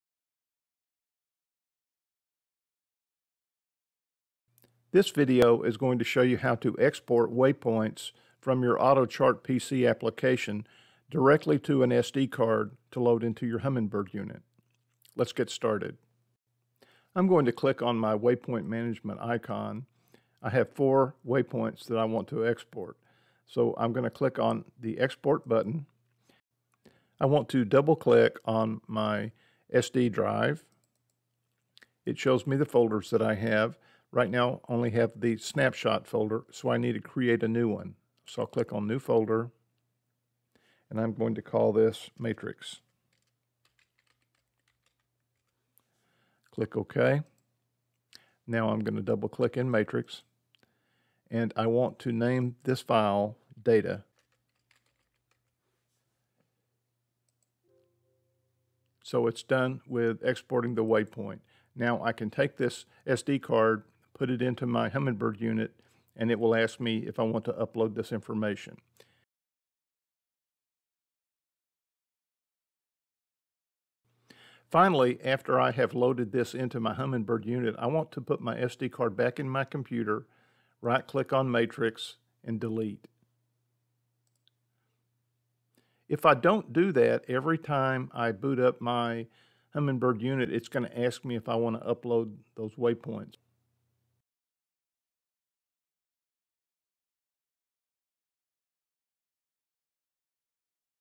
Export Waypoints from Auto Chart PC to SD Card directly
Skip Humminbird PC as the way to export your waypoints while using Auto Chart PC.  This tutorial shows you step-by-step how to do it.